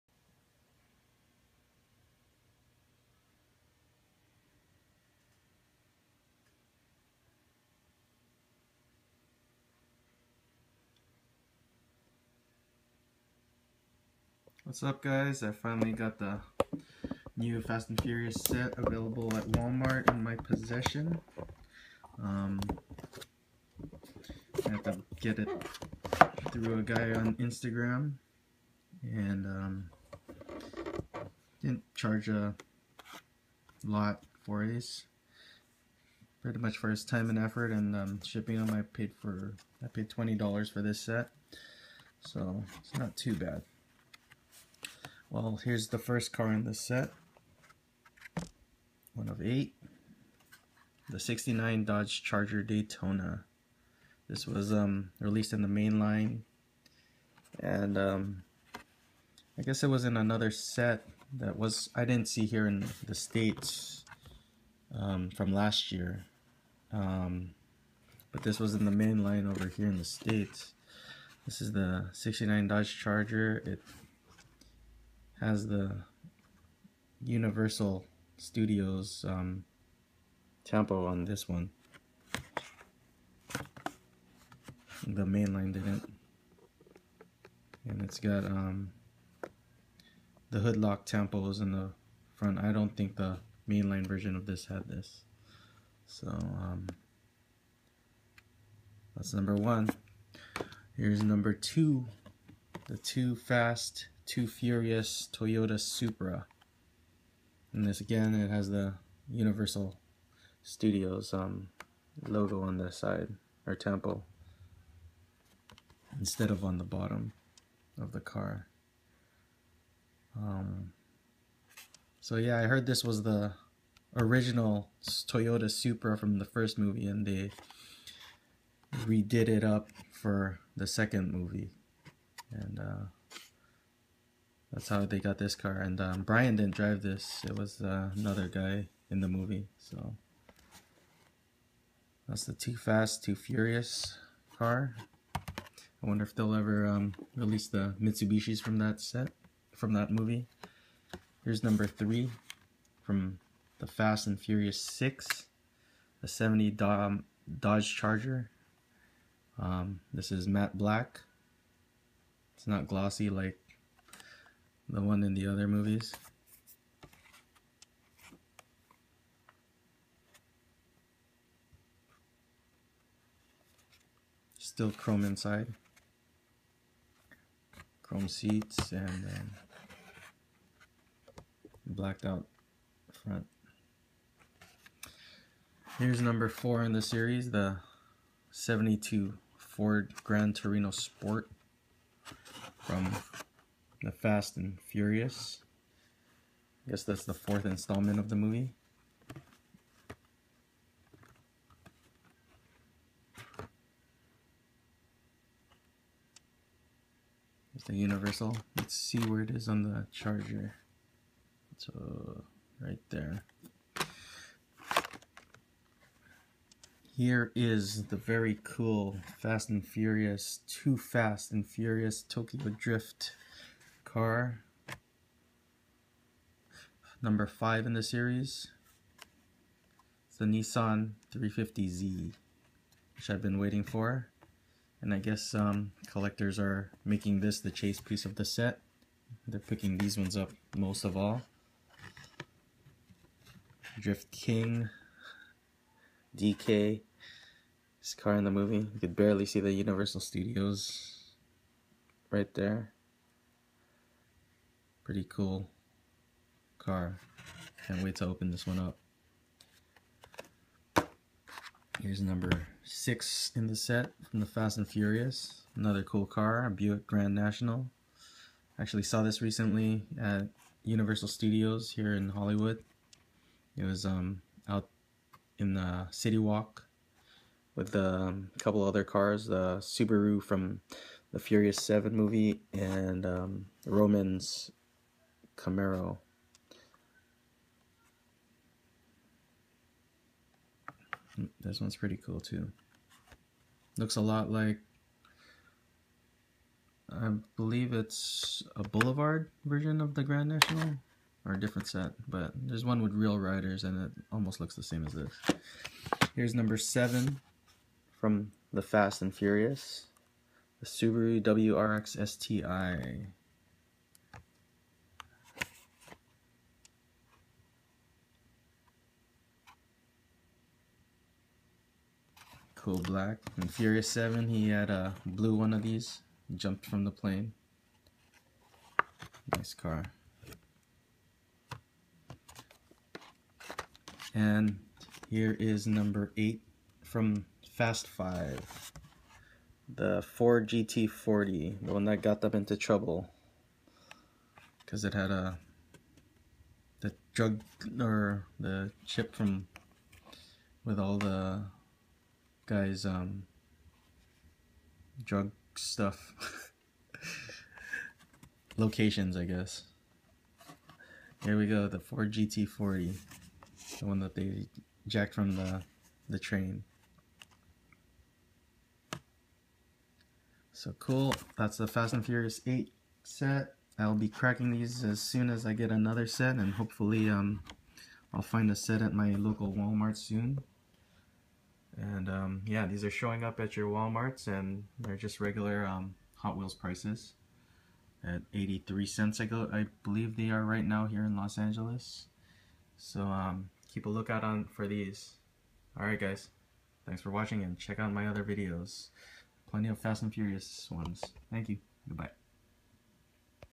2015 The Fast and Furious Hot Wheels 8-car set Exclusively from Walmart
I finally have the new Hot Wheels Fast & Furious set that is found exclusively at Walmart stores.  This set has some cool cars in the set.

Here is the list of cars:
1/8 - '69 Dodge Charger Daytona - Fast & Furious 6
2/8 - '94 Toyota Supra - 2 Fast 2 Furious
3/8 - '70 Dodge Charger R/T - Fast & Furious 6
4/8 - '72 Grand Torino Sport - Fast & Furious (4)
5/8 - Nissan 350Z - The Fast and the Furious: Tokyo Drift
6/8 - Buick Grand National - Fast & Furious (4)
7/8 - Subaru WRX STI - Fast & Furious (4)
8/8 - Ford GT40 - Fast Five